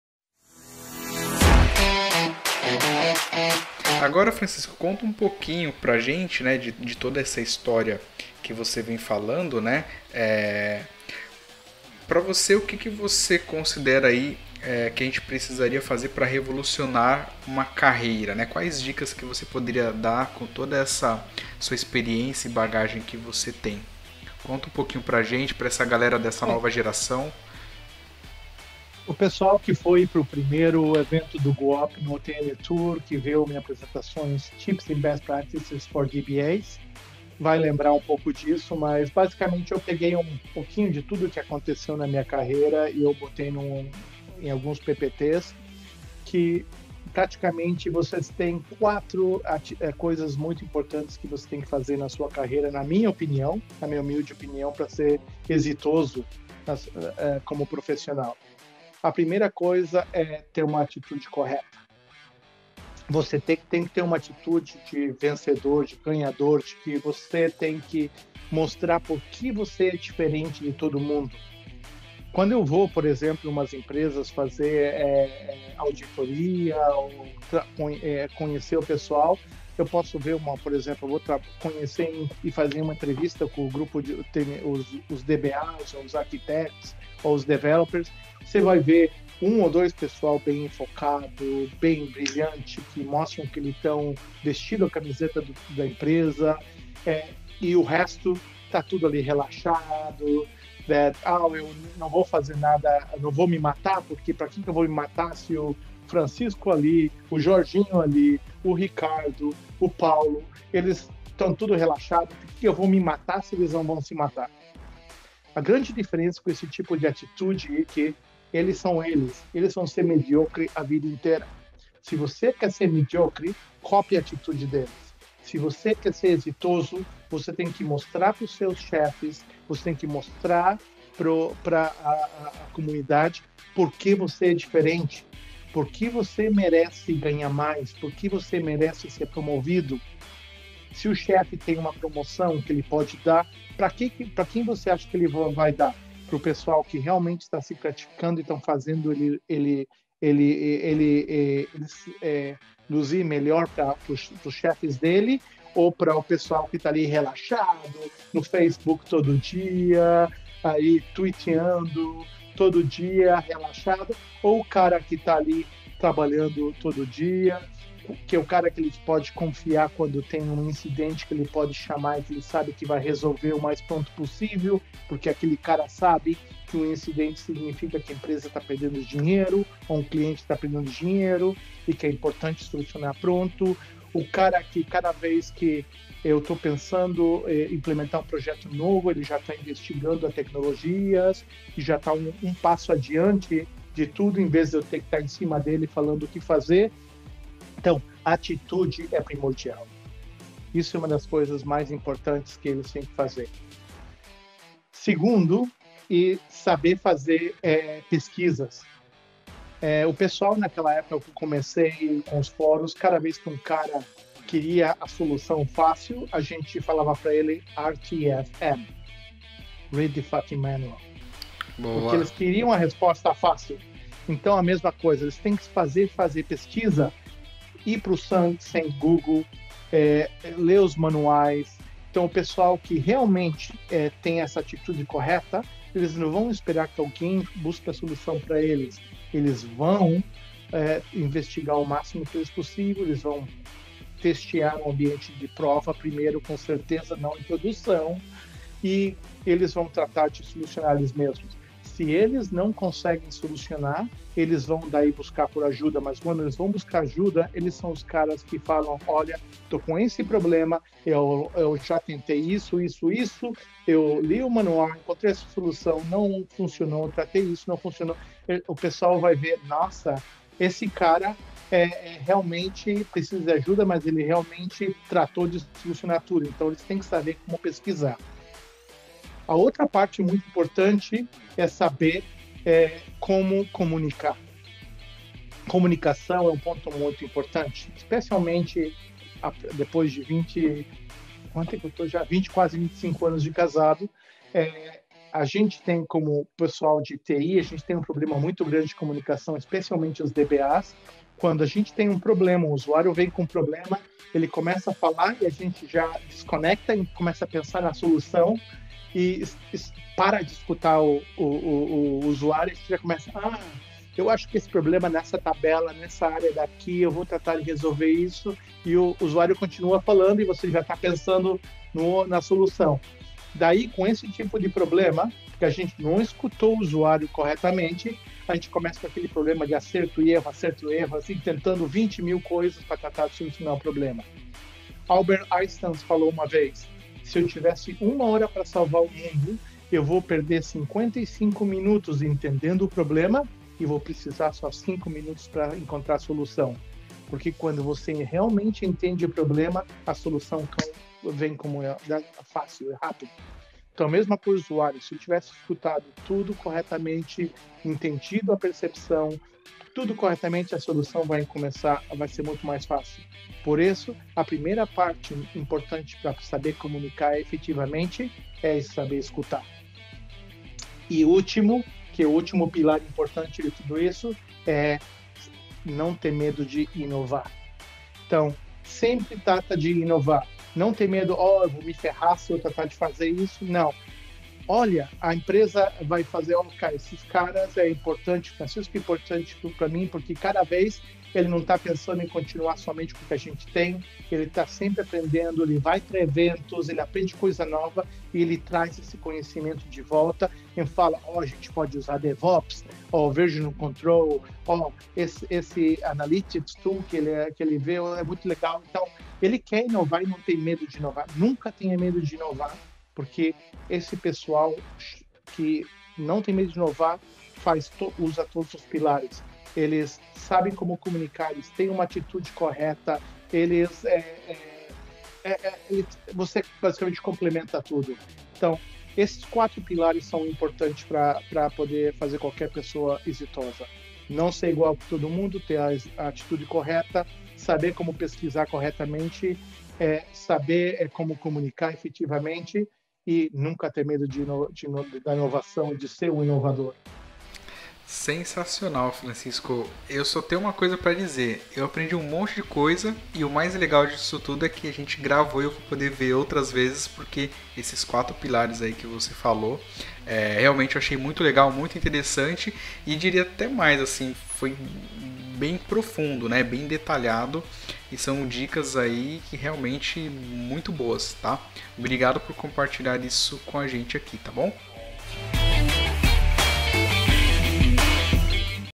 GGBR - Cortes - Francisco Alvarez: VOU TE MOSTRAR OS PILARES PRA VOCÊ REVOLUCIONAR A SUA CARREIRA...
GoldenTalks - Episódio 46: LIVE com Francisco Alvarez - Founder e CEO da CloudDB